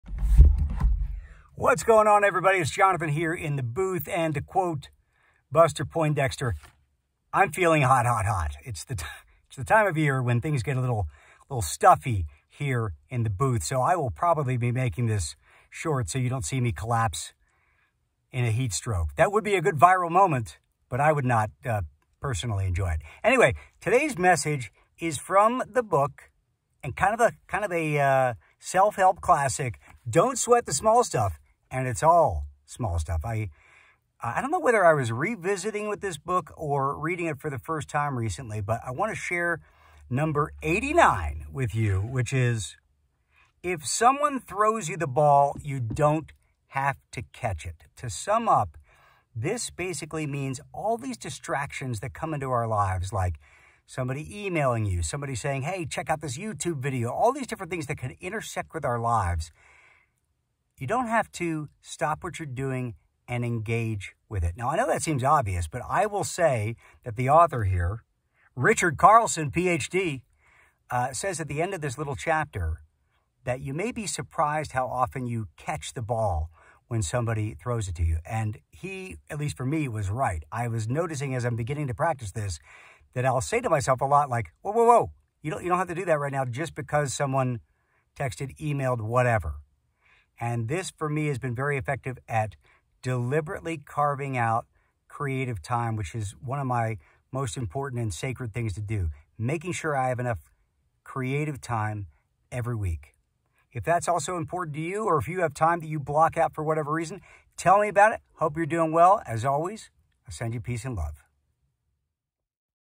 Don't catch everything thrown in your direction - Greetings from the Booth July 2023
I've been re-reading Don't Sweat the Small Stuff, and this passage about not having to catch every ball thrown your way really stuck out to me. It's important to carve out the time to do what you want to do.